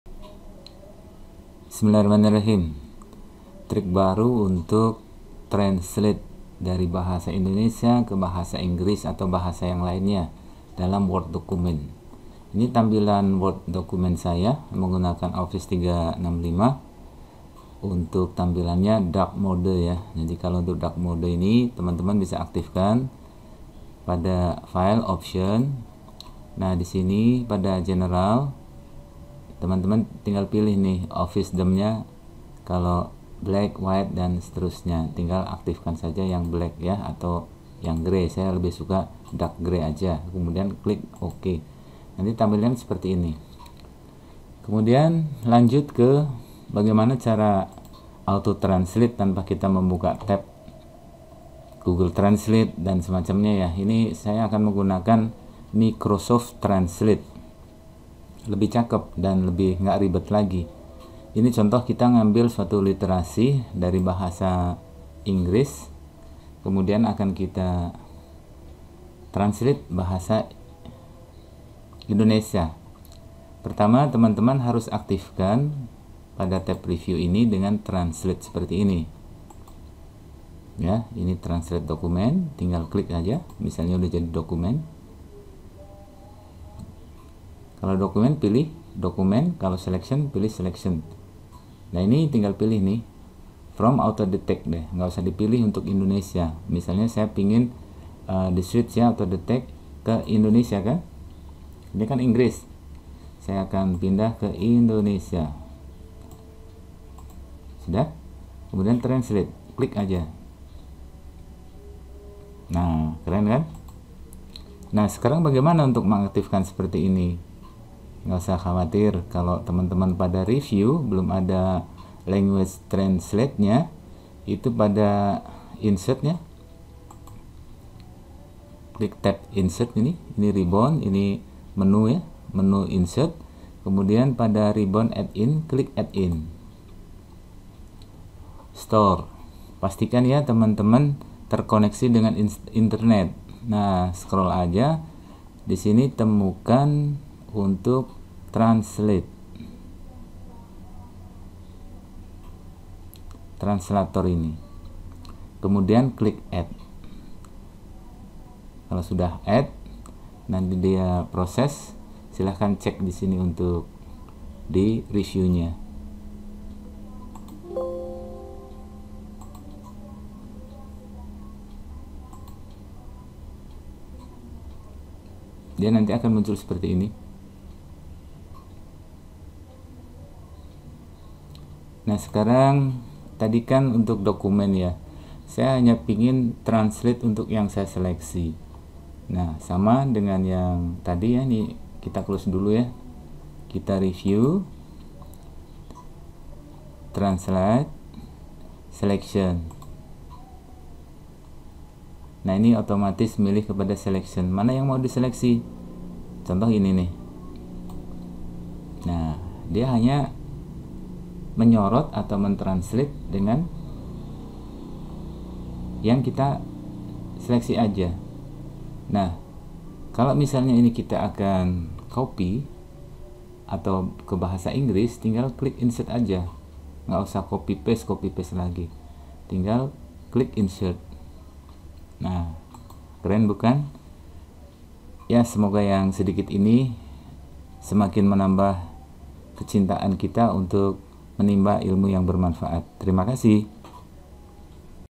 Cara cepat translate semua bahasa ke bahasa Indonesia tanpa harus edit lagi pada Word Document
Dengan Microsoft Office Translator pekerjaan translate bahasa menjadi semakin mudah banget